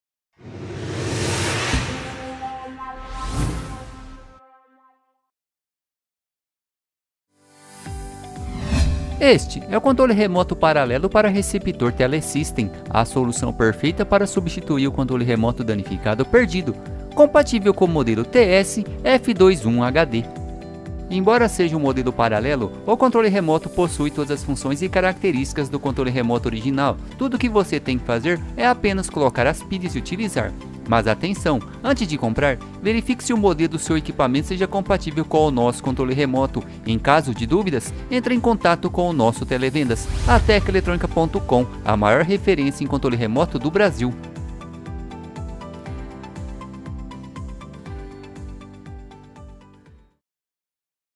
Controle Remoto Tele System C01203 (780169)
Controle Remoto para Receptor Tele System

Modelos Compatíveis: TS-F21 HD / TS-2300 HD

Embalagem: 01 Unidade

Garantia: 90 dias

Fabricante: Produto Importado